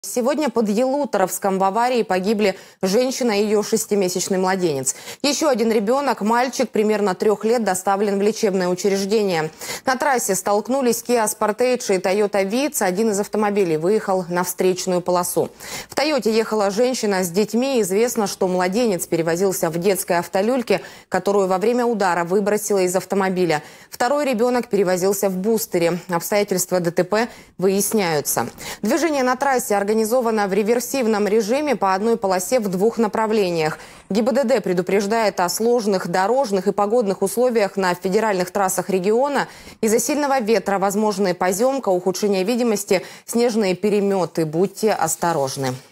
Трагедия на трассе | Ялуторовский район
12 января под Ялуторовском в аварии погибли женщина и её шестимесячный младенец. Ещё один ребёнок — мальчик примерно 3 лет — доставлен в лечебное учреждение. На трассе столкнулись "Киа Спортейдж" и "Тойота Витц", один из автомобилей выехал на встречную полосу. В "Тойоте" ехала женщина с детьми. Известно, что младенец перевозился в детской автолюльке, которую во время удара выбросило из автомобиля. Второй ребёнок перевозился в бустере. Обстоятельства ДТП выясняются.